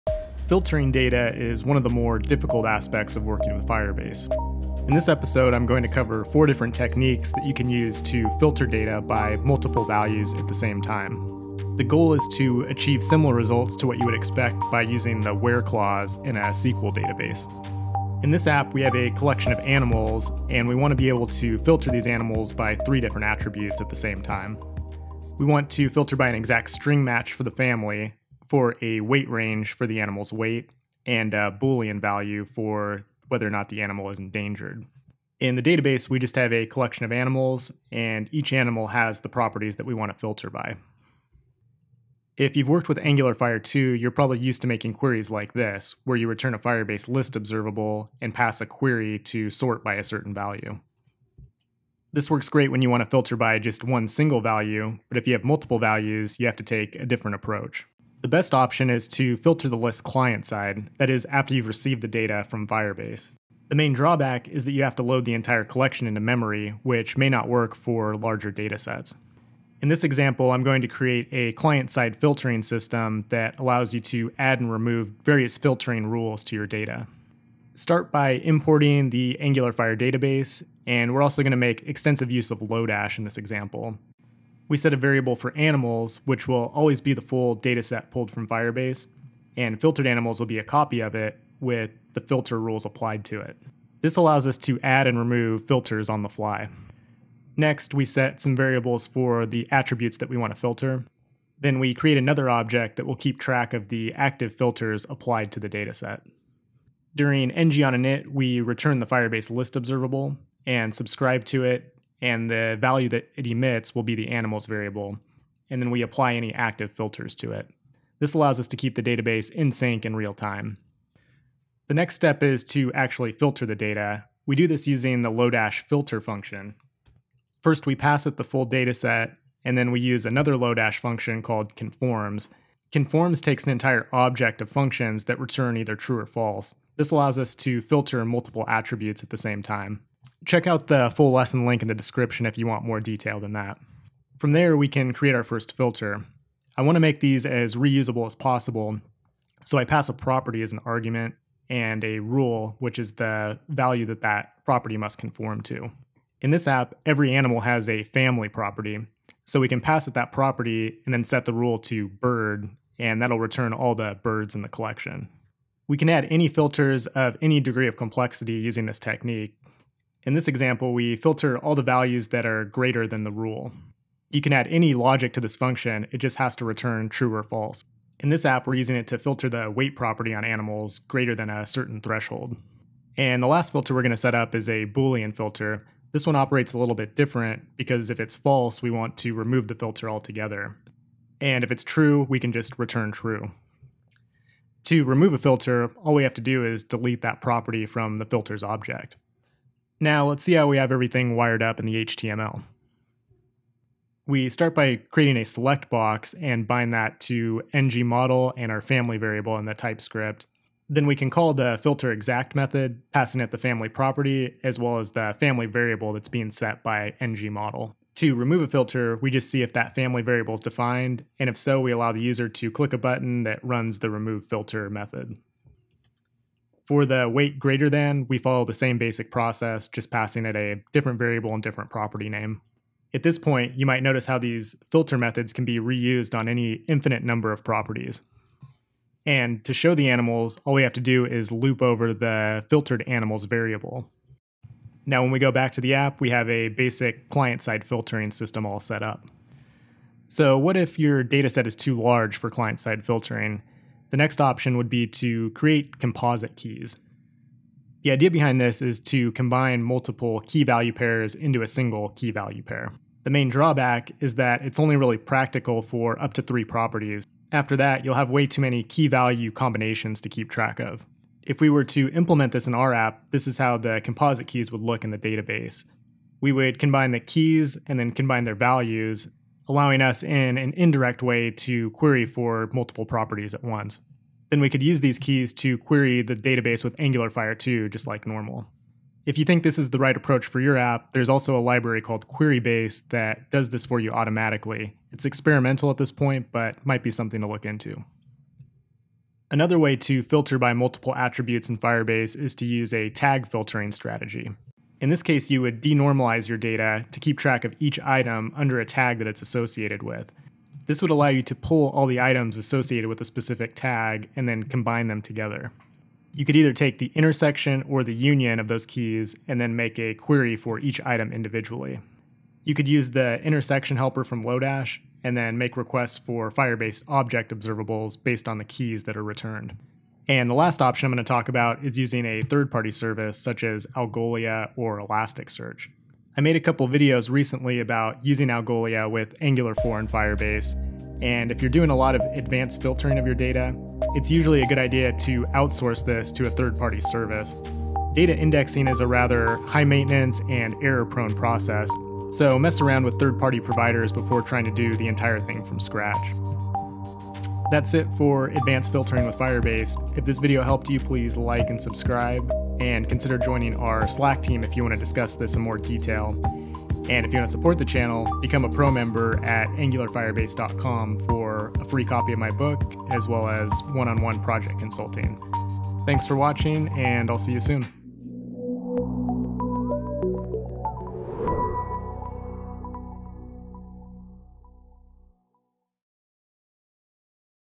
Advanced Firebase Data Filtering (Multi-Property)
In this episode I provide strategies for filtering data in Firebase with multiple properties or attributes, similar to a SQL where clause. Client-side filtering is performed with Angular 4.3. Check out the full lesson